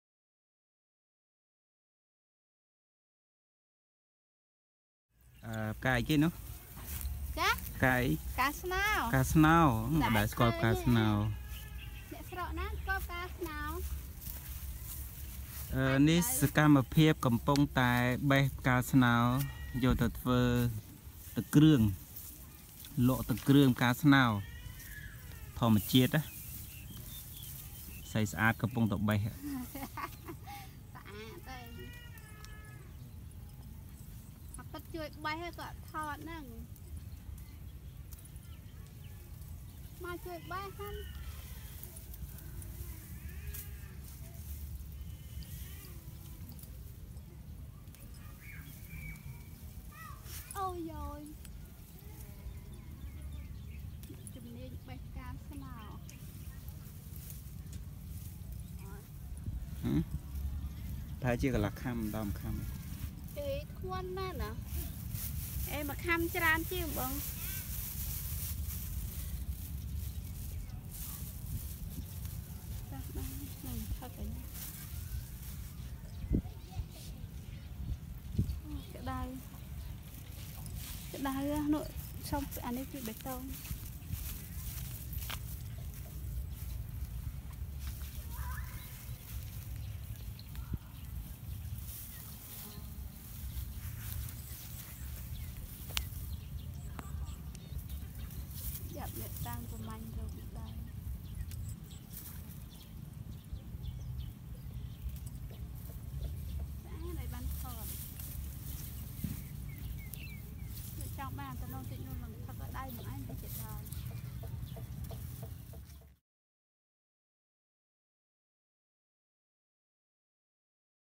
បេះផ្កាស្នោ Pka Snor Pick
បេះផ្កាស្នោយកមកធ្វើម្ហូប ទឹកគ្រឿង  ផ្កាស្នោដុះតាមត្រពាំង តាមធម្មជាតិ គ្មានអ្នកណាដាំទេ ។